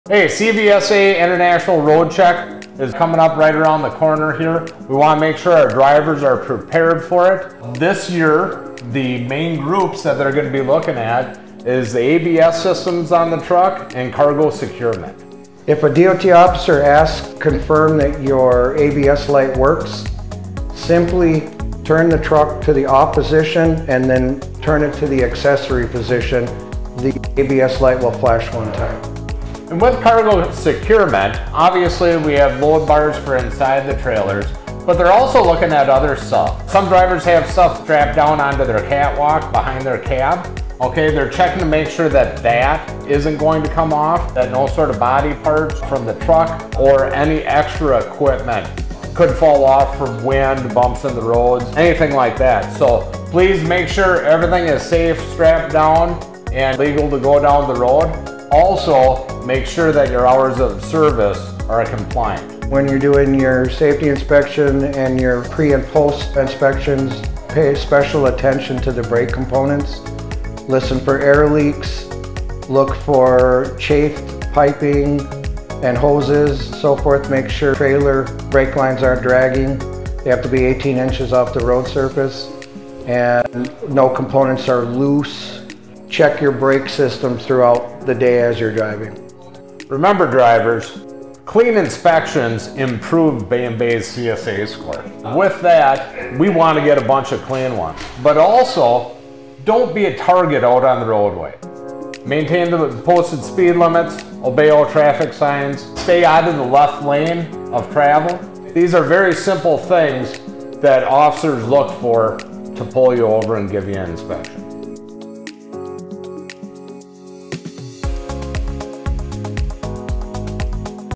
CVSA Roadcheck
Get ready for CVSA Roadcheck with insights and tips from Bay & Bay's Safety Team! Watch our latest video to learn some helpful tips and information from our experts to ensure a successful inspection. At Bay & Bay, safety is our top priority and our goal is to achieve zero violations. We're committed to ensuring the safety of our drivers and others on the road. Let's work together to make every journey a safe one!